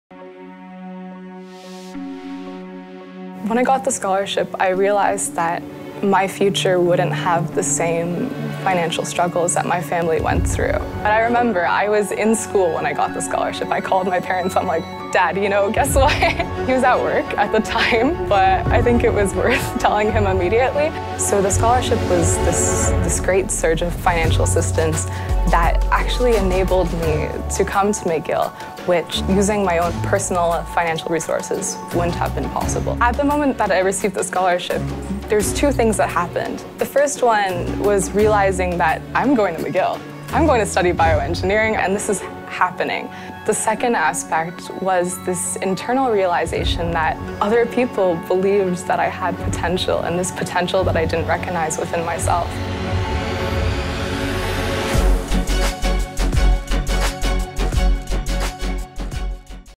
Scholarship support empowers aspiring engineer
Miasya Bulger’s passion for STEM started in high school. At sixteen, she was the only girl representing her school at programming competitions. But she wasn’t sure she’d be able to pursue her passion due to her family’s finances. Today, thanks to a Schulich Leader Scholarship, she is living out “one of the best things that ever happened” and studying Bioengineering at McGill.